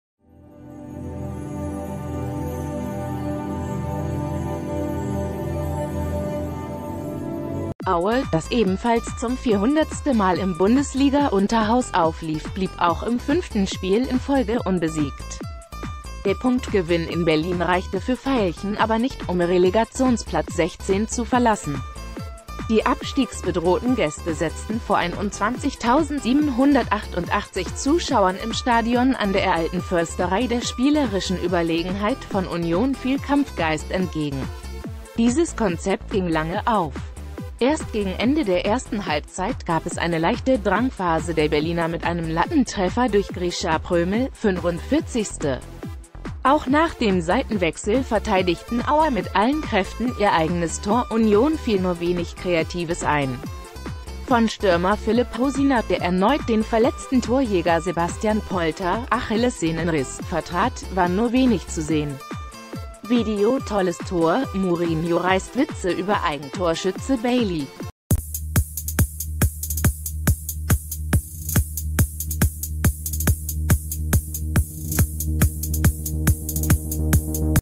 Aktuelle Nachrichten | Union Berlin holt im Jubiläumsspiel gegen Aue nur einen Punkt
Aue, das ebenfalls zum 400. Mal im Bundesliga-Unterhaus auflief, blieb auch im fünften Spiel in Folge unbesiegt. Der Punktgewinn in Berlin reichte für  Veilchen aber nicht, um Relegationsplatz 16 zu verlassen. Die abstiegsbedrohten Gäste setzten vor 21.788 Zuschauern im Stadion an der Alten Försterei der spielerischen Überlegenheit von Union viel Kampfgeist entgegen. Dieses Konzept ging lange auf. Erst gegen Ende der ersten Halbzeit gab es eine leichte Drangphase der Berliner mit einem Lattentre...